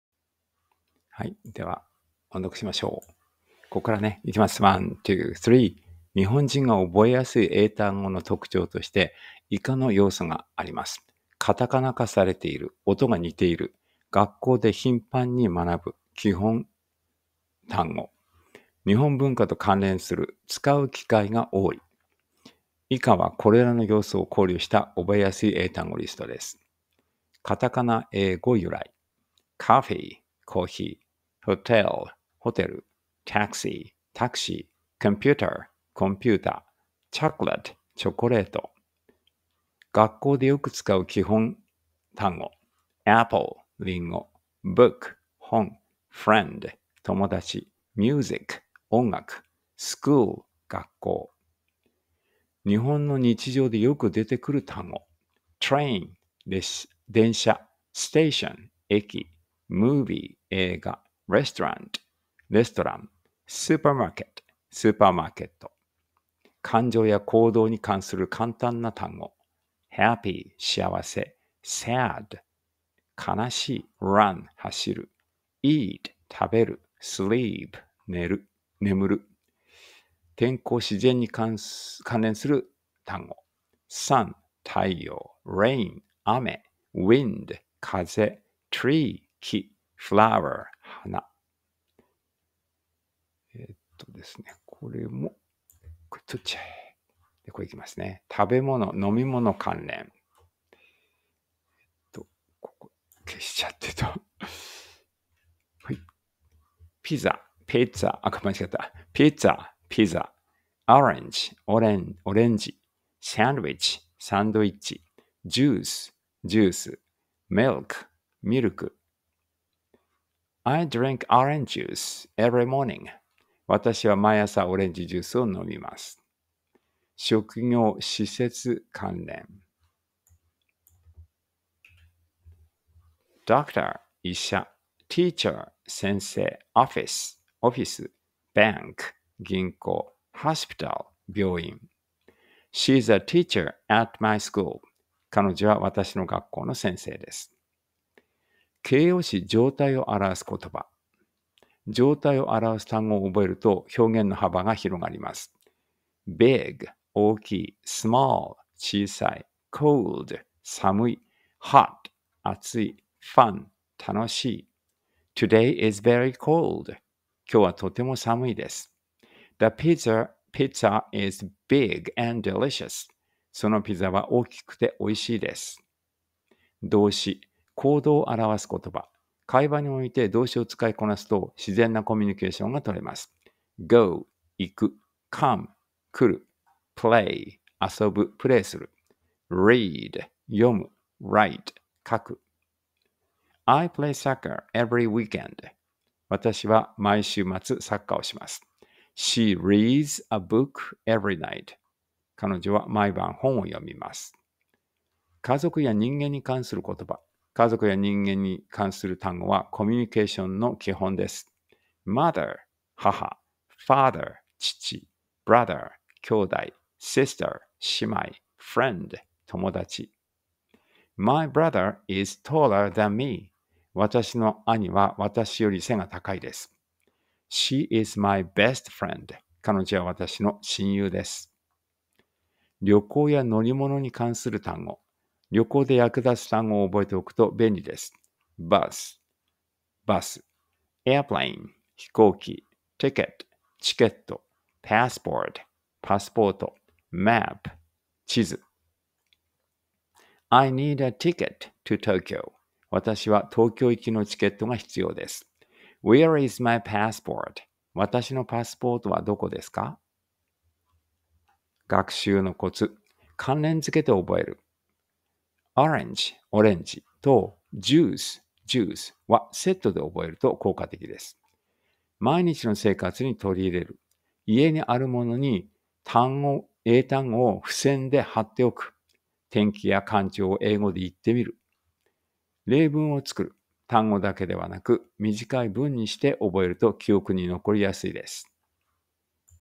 えいご おんどく 400
日本人が覚えやすい英単語の特徴として、以下の要素があります。
- **カタカナ化されている**（音が似ている）
- **学校で頻繁に学ぶ**（基本単語）
- **日本文化と関連する**（使う機会が多い）

以下は、これらの要素を考慮した覚えやすい英単語リストです。

### 1. **カタカナ英語由来**
- **coffee**（コーヒー）
- **hotel**（ホテル）
- **taxi**（タクシー）
- **computer**（コンピュータ）
- **chocolate**（チョコレート）

### 2. **学校でよく使う基本単語**
- **apple**（リンゴ）
- **book**（本）
- **friend**（友達）
- **music**（音楽）
- **school**（学校）

### 3. **日本の日常でよく出てくる単語**
- **train**（電車）
- **station**（駅）
- **movie**（映画）
- **restaurant**（レストラン）
- **supermarket**（スーパーマーケット）

### 4. **感情や行動に関する簡単な単語**
- **happy**（幸せ）
- **sad**（悲しい）
- **run**（走る）
- **eat**（食べる）
- **sleep**（眠る）

### 5. **天候・自然に関連する単語**
- **sun**（太陽）
- **rain**（雨）
- **wind**（風）
- **tree**（木）
- **flower**（花）

## 1. **食べ物・飲み物関連**
- **pizza**（ピザ）
- **orange**（オレンジ）
- **sandwich**（サンドイッチ）
- **juice**（ジュース）
- **milk**（ミルク）

### 例文
- I drink **orange juice** every morning.（私は毎朝オレンジジュースを飲みます。）

## 2. **職業・施設関連**

- **doctor**（医者）
- **teacher**（先生）
- **office**（オフィス）
- **bank**（銀行）
- **hospital**（病院）

### 例文
- She is a **teacher** at my school.（彼女は私の学校の先生です。）

## 3. **形容詞・状態を表す言葉**
「状態」を表す単語を覚えると、表現の幅が広がります。
- **big**（大きい）
- **small**（小さい）
- **cold**（寒い）
- **hot**（暑い）
- **fun**（楽しい）

### 例文
- Today is very **cold**.（今日はとても寒いです。）
- The pizza is **big** and delicious.（そのピザは大きくて美味しいです。）

## 4. **動詞：行動を表す言葉**
会話において動詞を使いこなすと、自然なコミュニケーションが取れます。
- **go**（行く）
- **come**（来る）
- **play**（遊ぶ、プレイする）
- **read**（読む）
- **write**（書く）

### 例文
- I **play** soccer every weekend.（私は毎週末サッカーをします。）
- She **reads** a book every night.（彼女は毎晩本を読みます。）

## 5. **家族や人に関する言葉**
家族や人に関する単語は、コミュニケーションの基本です。
- **mother**（母）
- **father**（父）
- **brother**（兄弟）
- **sister**（姉妹）
- **friend**（友達）

### 例文
- My **brother** is taller than me.（私の兄は私より背が高いです。）
- She is my best **friend**.（彼女は私の親友です。）

## 6. **旅行や乗り物に関する単語**
旅行で役立つ単語を覚えておくと便利です。
- **bus**（バス）
- **airplane**（飛行機）
- **ticket**（チケット）
- **passport**（パスポート）
- **map**（地図）

### 例文
- I need a **ticket** to Tokyo.（私は東京行きのチケットが必要です。）
- Where is my **passport**?（私のパスポートはどこですか？）

### **学習のコツ**
1. **関連付けて覚える**
   - 例：*「orange（オレンジ）」と「juice（ジュース）」*はセットで覚えると効果的です。
2. **毎日の生活に取り入れる**
   - 家にあるものに英単語を付箋で貼っておく。
   - 天気や感情を英語で言ってみる。
3. **例文を作る**
   - 単語だけではなく、短い文にして覚えると記憶に残りやすいです。